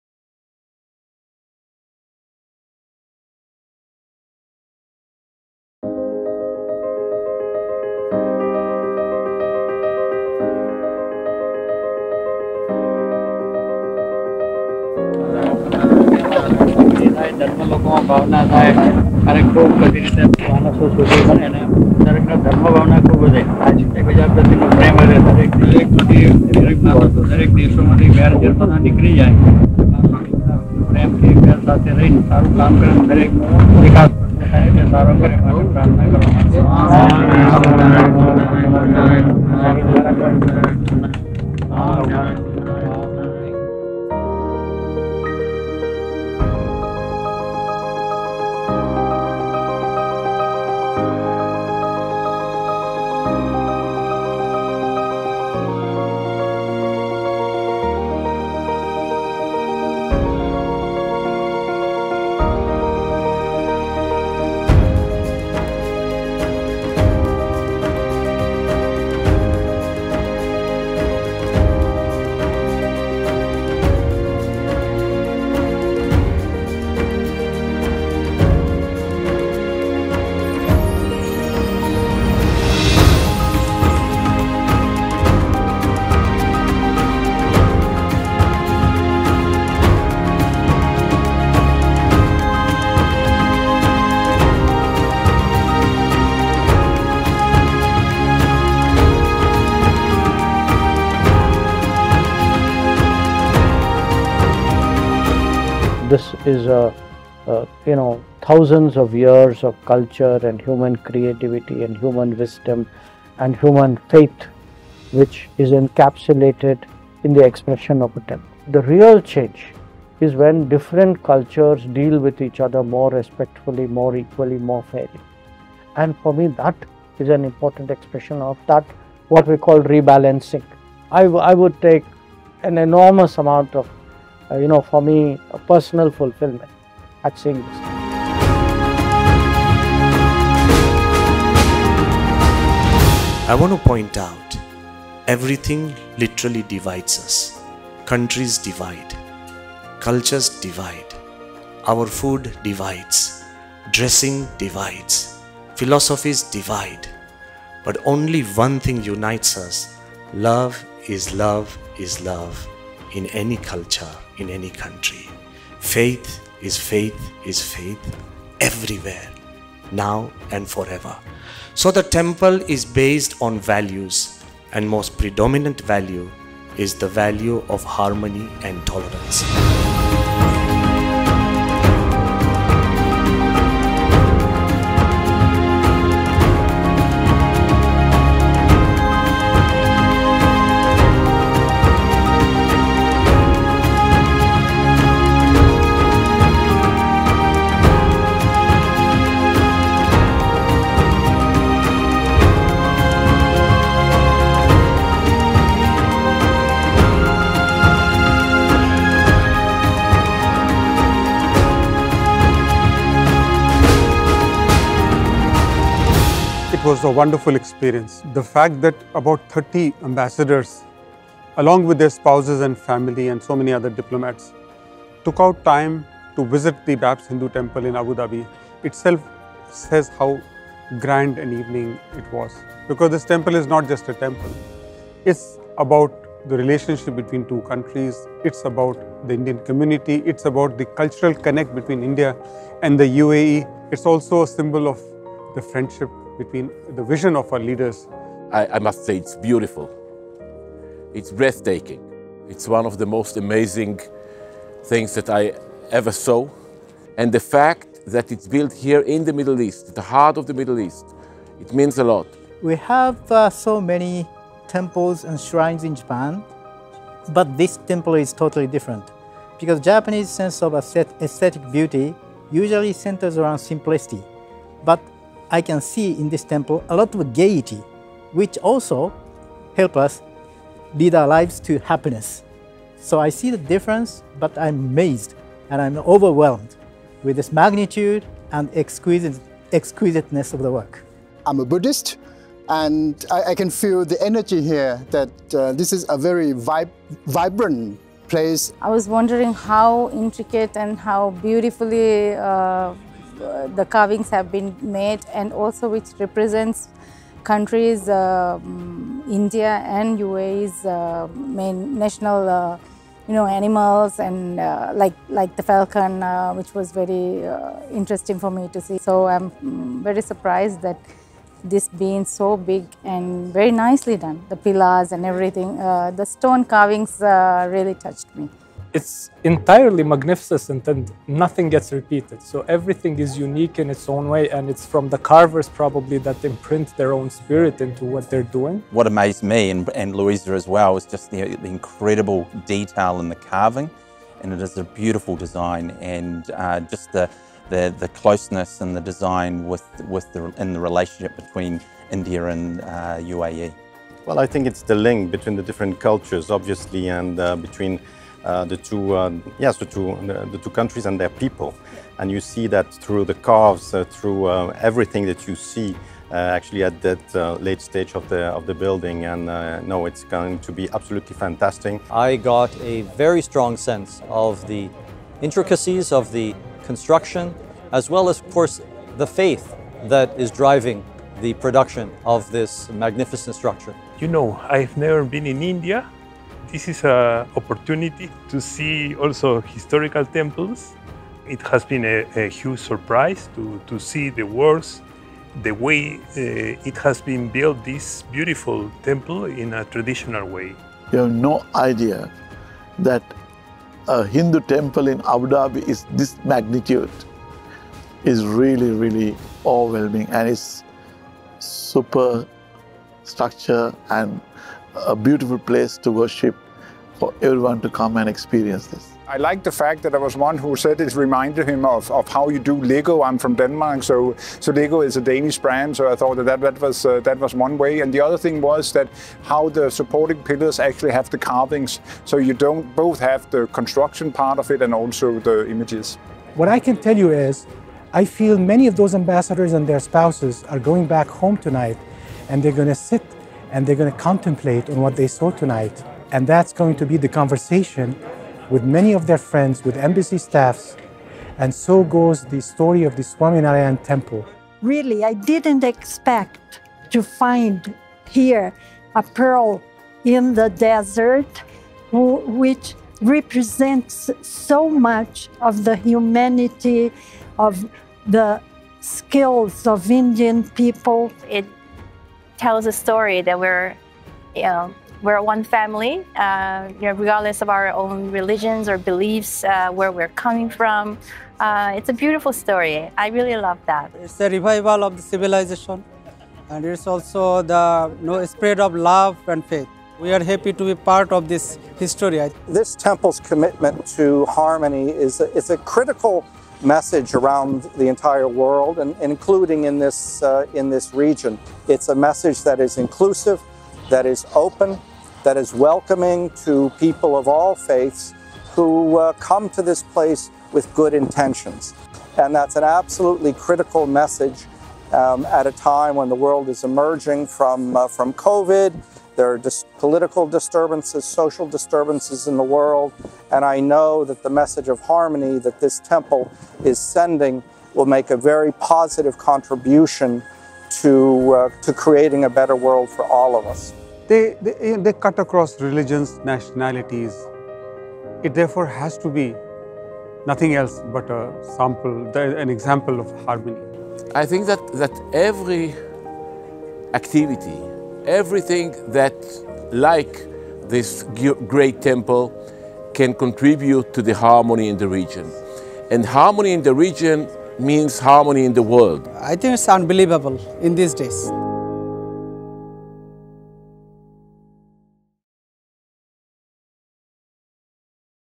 Ambassadors from over 30 Nations celebrate Global Harmony at BAPS Hindu Mandir, Abu Dhabi
In a celebration of global harmony, Ambassadors from over 30 nations gathered at the BAPS Hindu Mandir in Abu Dhabi. This momentous occasion showcased the power of unity and intercultural understanding as they embarked on a journey of shared values. In his address, Swami Brahmaviharidas warmly welcomed the Ambassadors and their families, emphasizing the importance of "Celebrating the past, addressing the present, and recalibrating the future." The ambassadors were inspired by the mandir's historical significance and its embodiment of universal values.This video captures the experiences and opinions of the ambassadors.